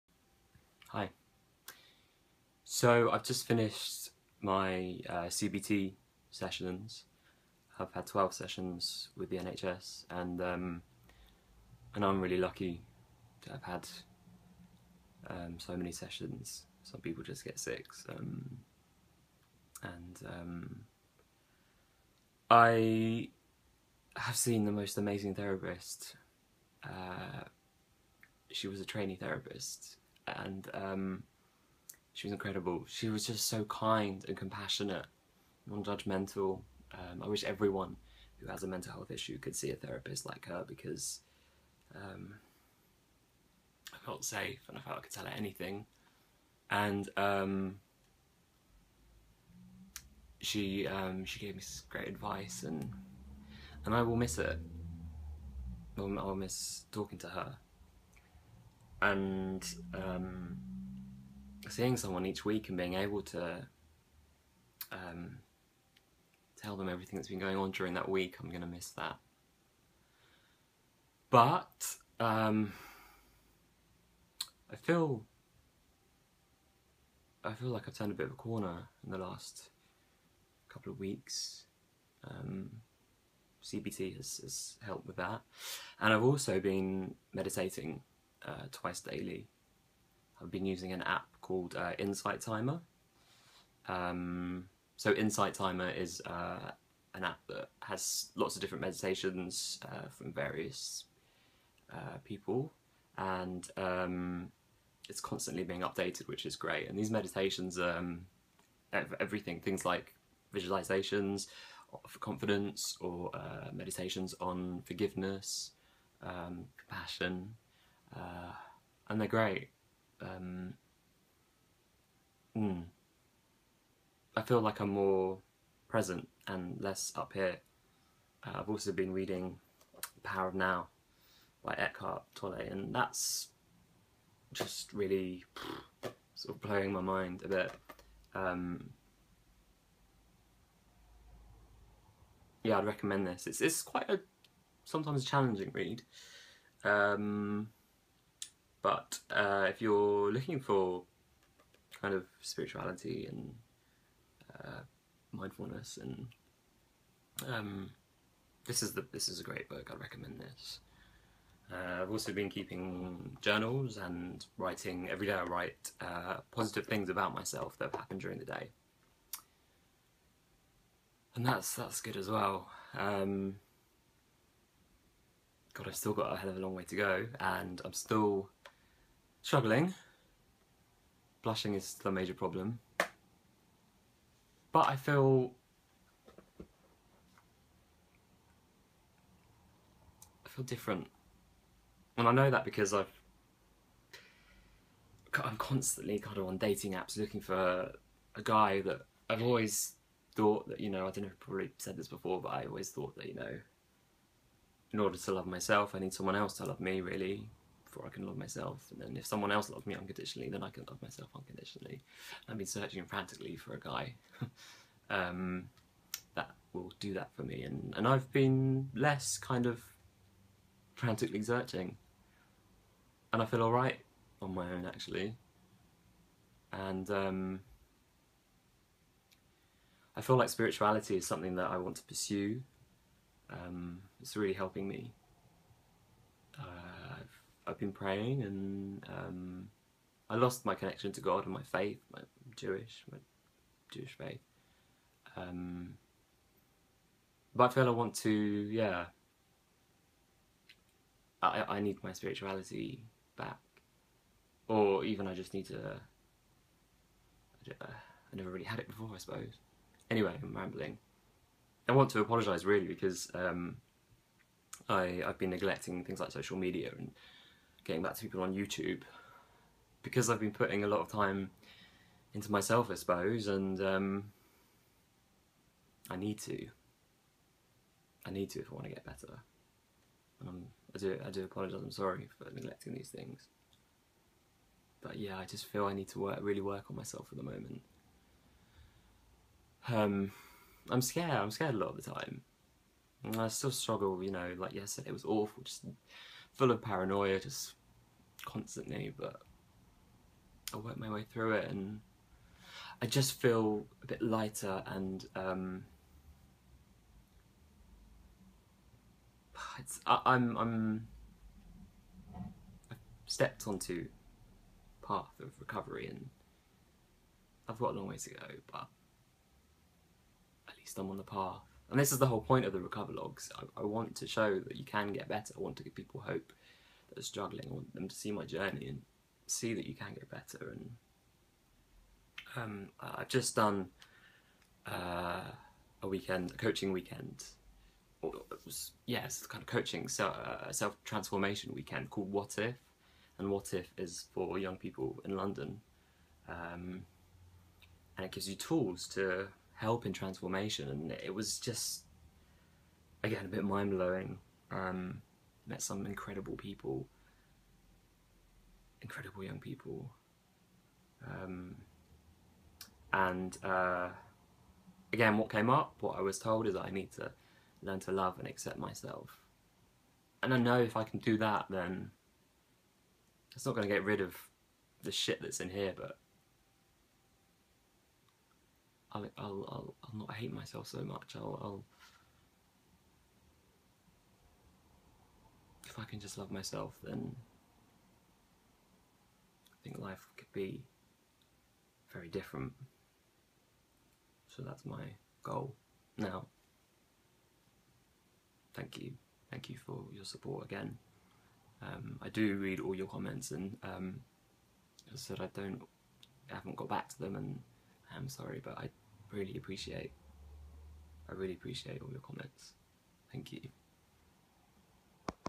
Recoverlog 19: Turning A Corner (At last (: )
I'm creating a video diary to chart my recovery from mental illness.

I hope this will help other people who are also struggling with their mental health to speak out, get help and recover.